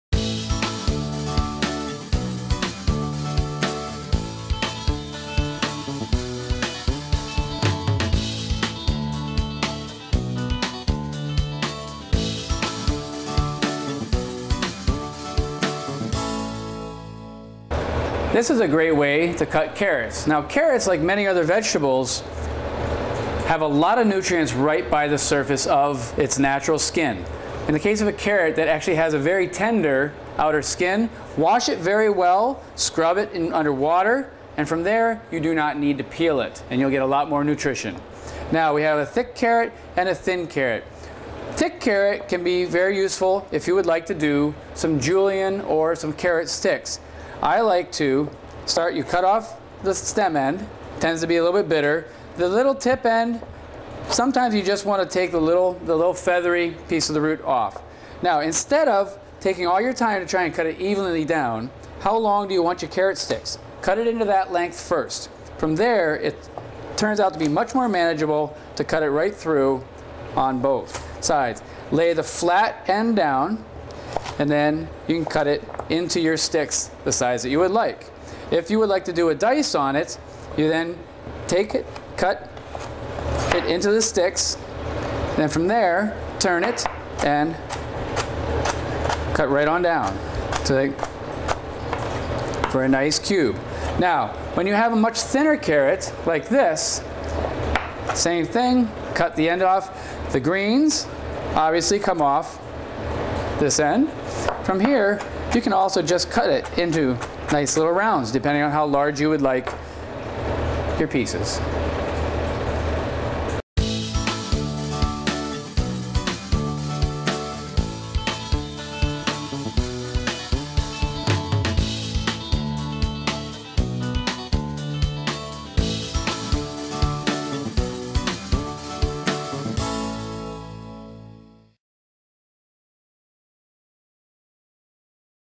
Cleaning, slicing, chopping fresh carrots -- Peter Robertson, RPs Pasta, Lubing Creative
Peter Roberson of RP's Pasta Company, in Madison, WI shows us knife skills on carrots from the Dane County Farmers' Market.